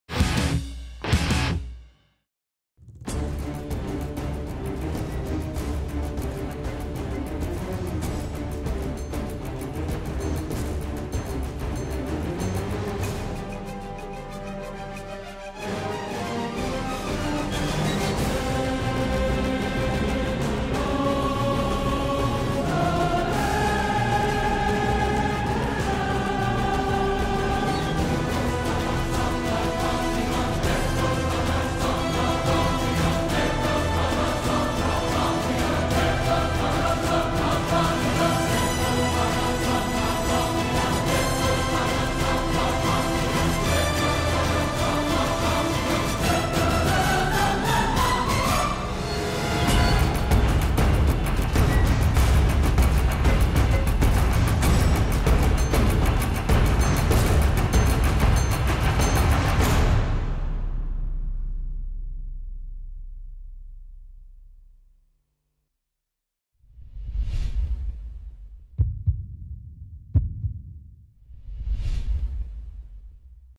Teaser Dragons vs Bradford
14ème journée de Super League
Dragons Catalans vs Bradford
Samedi 24 Mai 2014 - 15h - Stade Gilbert Brutus - Perpignan